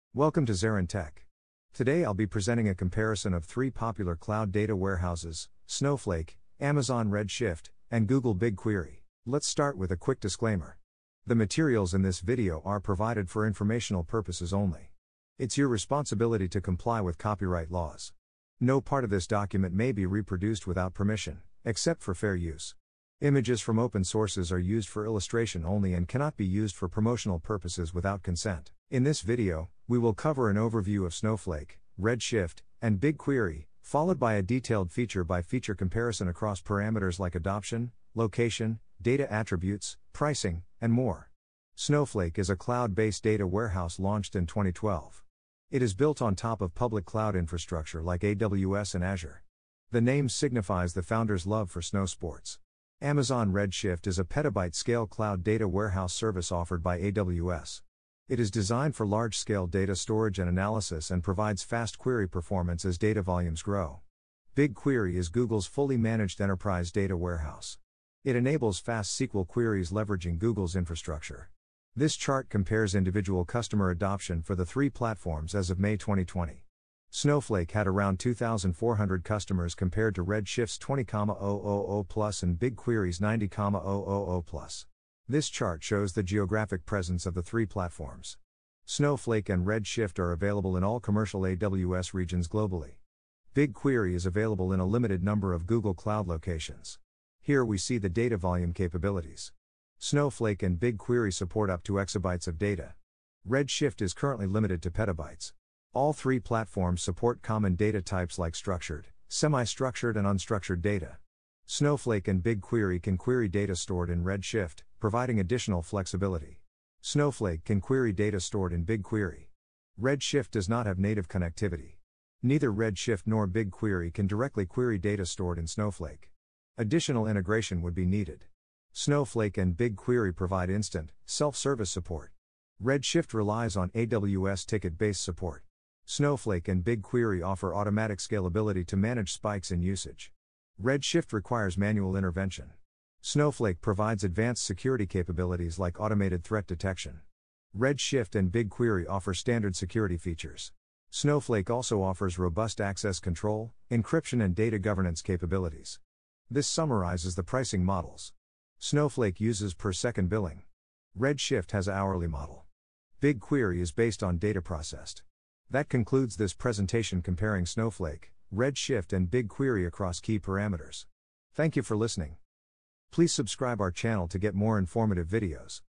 Snowflake Vs. Amazon Redshift Vs. Google Big Query | ZaranTech
👉 In this video you will learn about Snowflake Vs. Amazon Redshift Vs. Google Big Query.